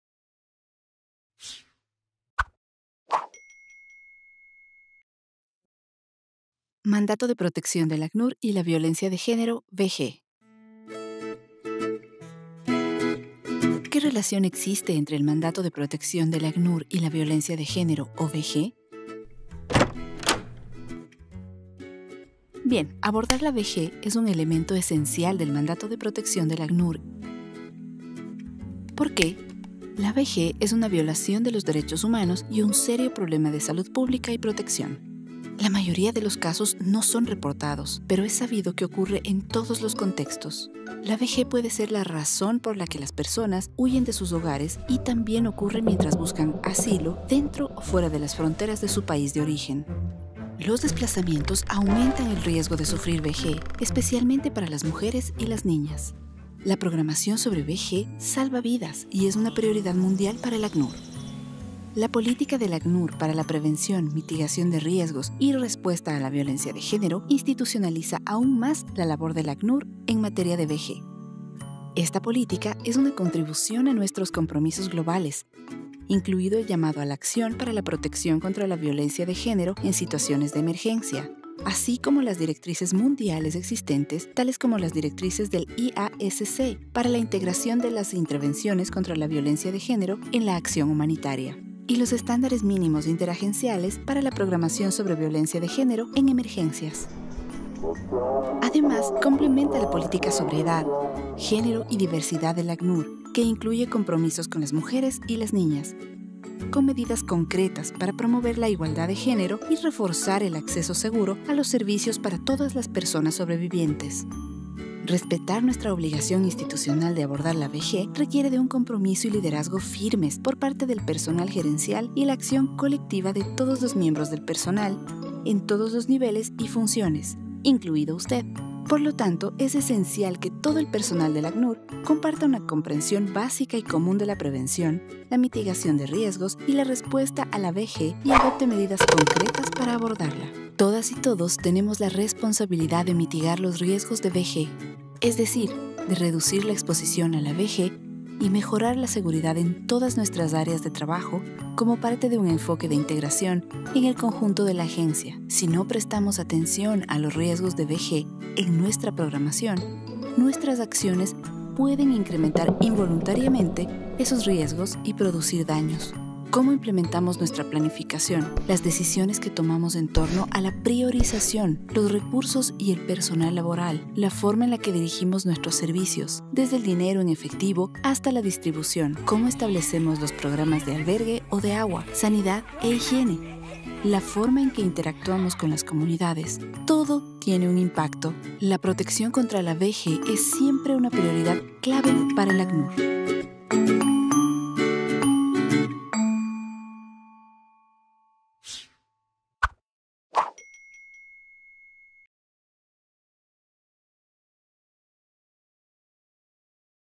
Mandato de Protección del ACNUR y la Violencia de Género
This short, animated video will help you explain why Gender-based Violence (GBV) is a core component of UNHCR’s protection mandate.

Remember:

GBV is a core component of UNHCR’s Protection Mandate.

GBV is a human rights violation and a major public health issue.

Displacement increases the risk of GBV.

GBV programming is lifesaving and an institutional priority.

For further reference please see:

UNHCR Policy on the Prevention of, Risk Mitigation, and Response to GBV (2020)

The IASC GBV Guidelines for Integrating GBV Intervention in Humanitarian Action (2015)